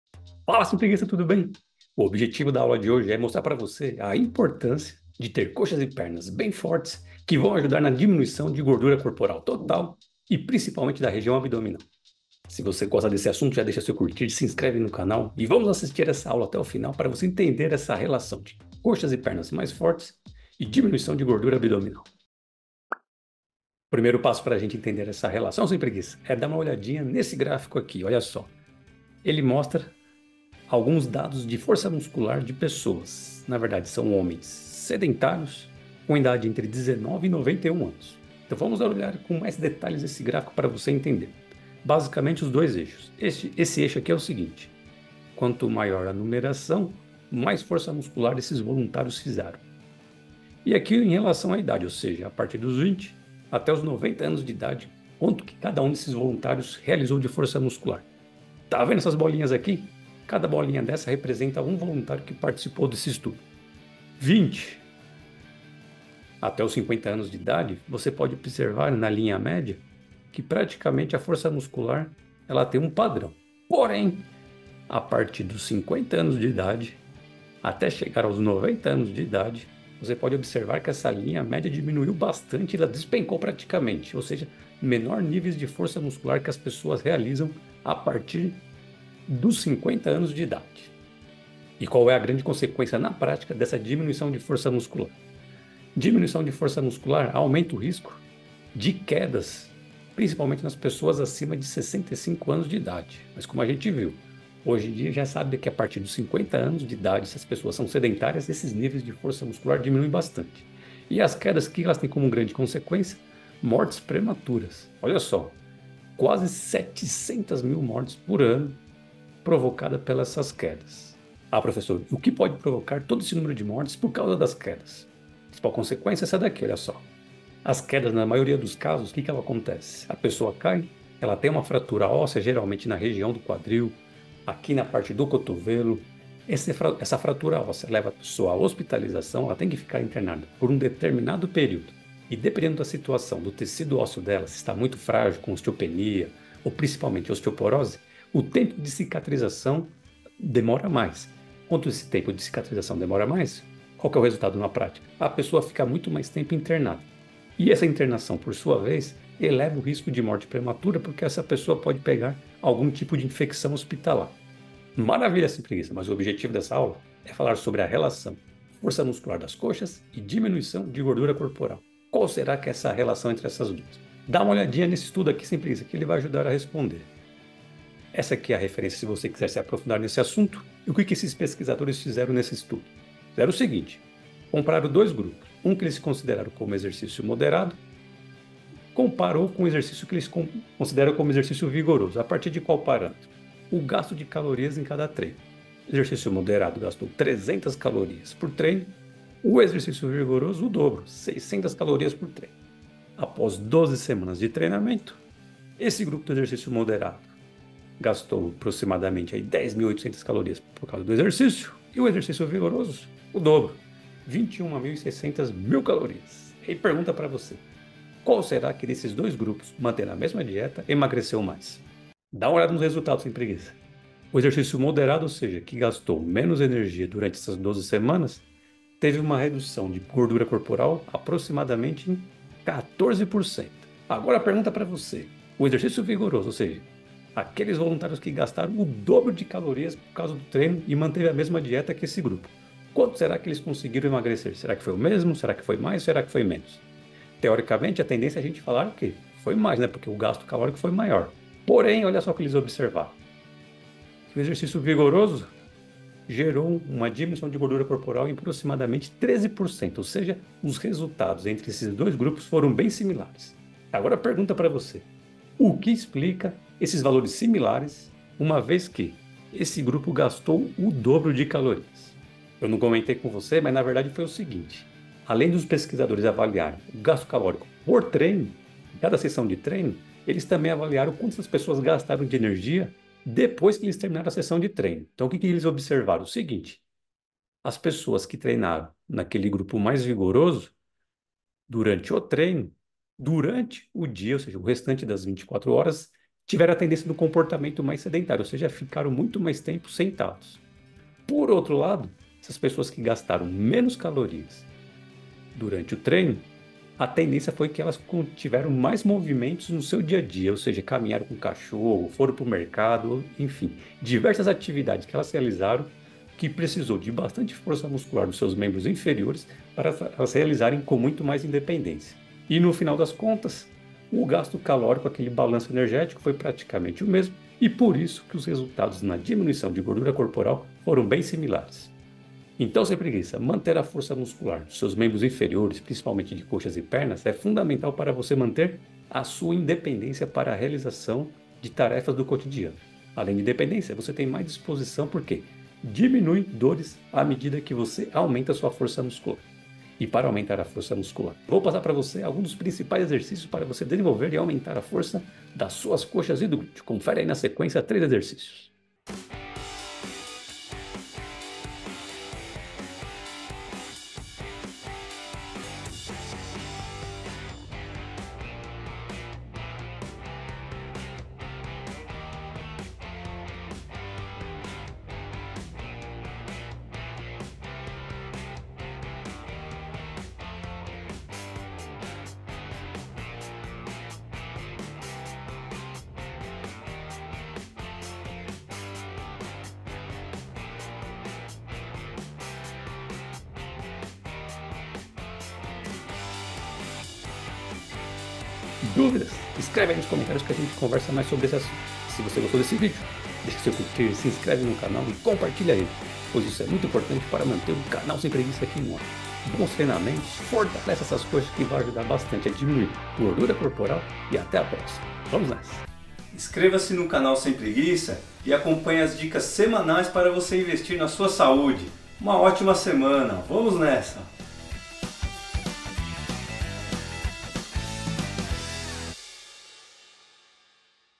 POR QUE é importante treinar COXAS para DIMINUIR GORDURA ABDOMINAL?
Veja a importância de fazer musculação para aumentar a força das coxas e do glúteo na diminuição de gordura abdominal. Além disso, veja 3 exercícios de musculação que eu recomendo para deixar suas coxas mais fortes.

E-book Definição abdominal: Saúde ou estética?
Baixe grátis!